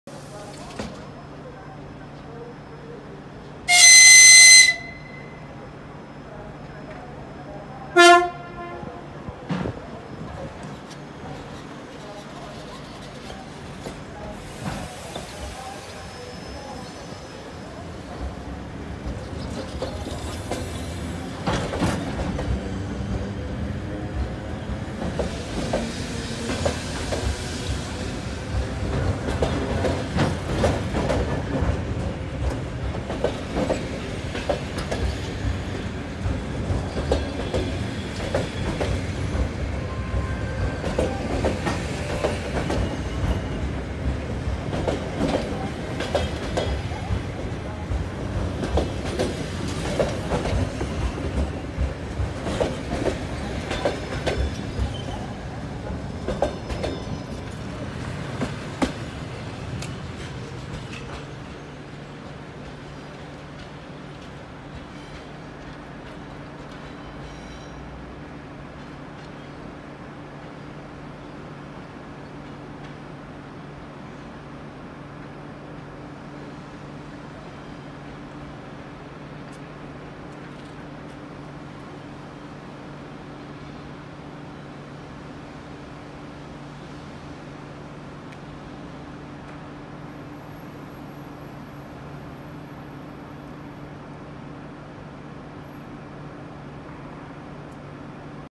ЭД4М-0307 На Курском вокзале
ОПИСАНИЕ

С завода поступил в депо Железнодорожная Московской ж. д.

Сетевые коды:
01 – 13454103
02 – 13454111
03 – 13454129
04 – 13454137
05 – 13454145
06 – 13454152
07 – 13454160
08 – 13454178
09 – 13454186
10 – 13454194
11 – 13454202

06.2017 — КР-1 Московский ЛРЗ
02.2021 – ТР-3